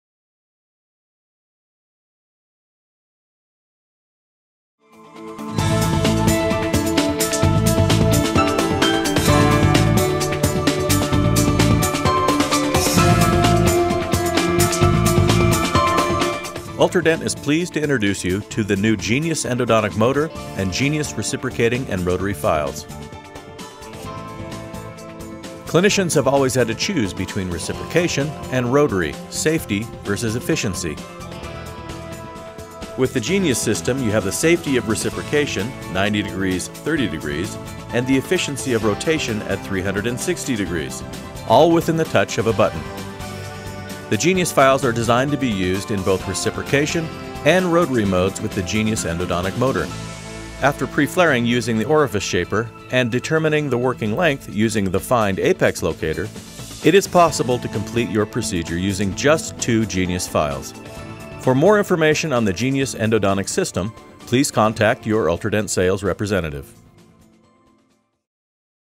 Genius Endodontic Motor and File Introduction video
This video will provide an introduction to the Genius Endodontic Motor and file system.  It is designed as a short introduction which will encourage the customer to get more information on the system.